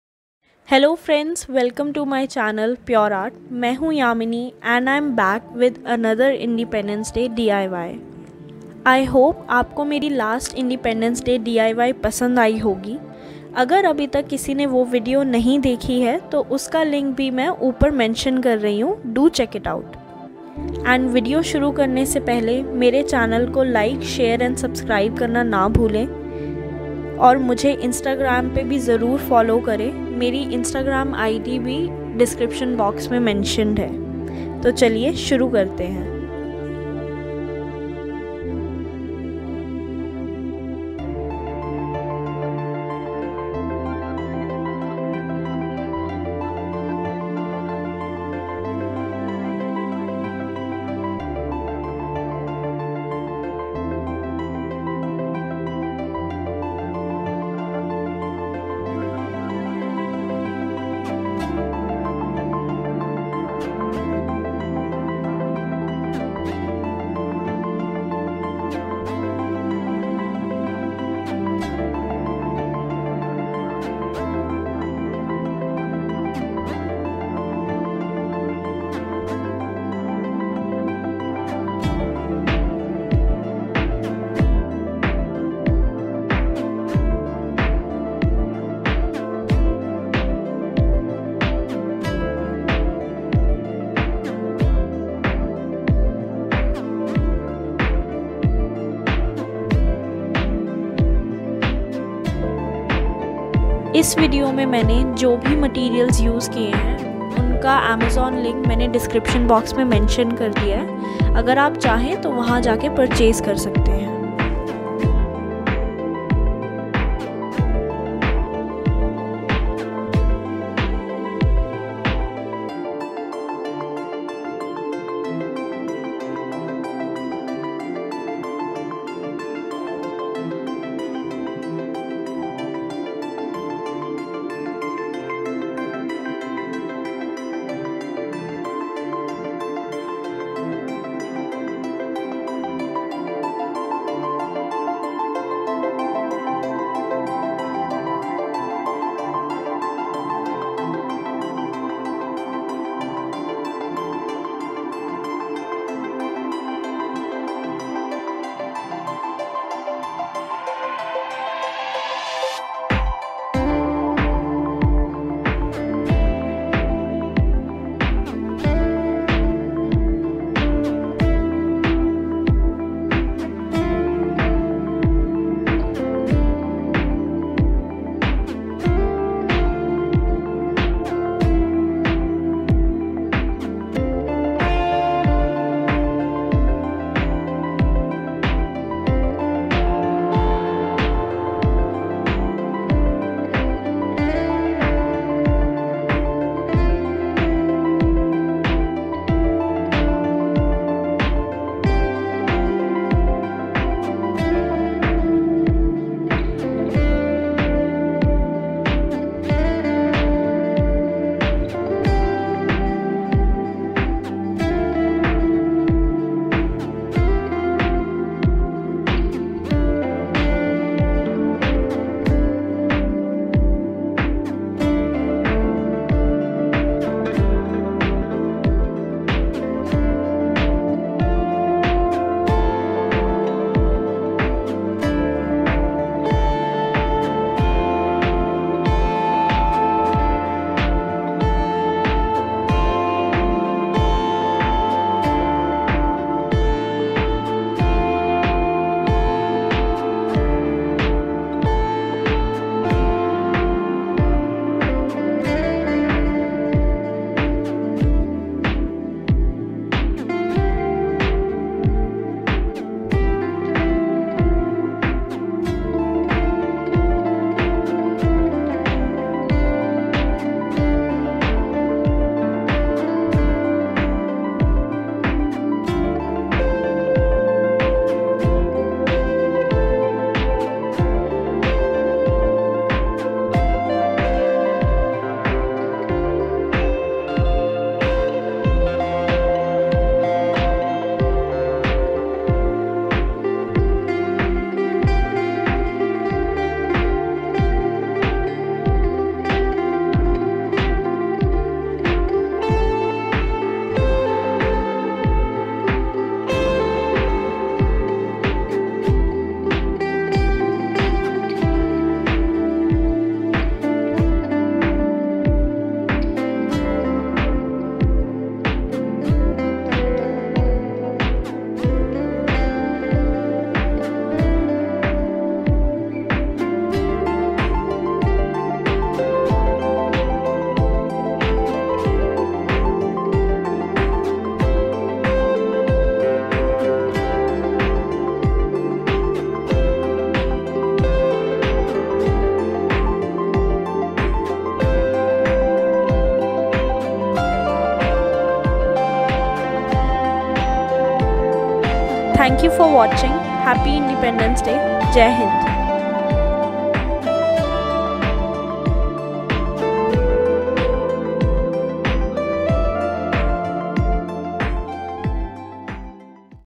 Independence Day Craft | Mandala Art using Brush pens
Materials used in this video:
1. Camlin Kokuyo Brush Pens, 24 Shades (Multicolor)
2. White Gelly Roll Pen

Today we are making an art project to celebrate the 15th of August!
We're going to make mandala art using brush pens.
I'll show you how to draw a pattern for a mandala, and then how to color it in.
This is such a fun project that anyone can do - even if you don't consider yourself to be very artistic or creative.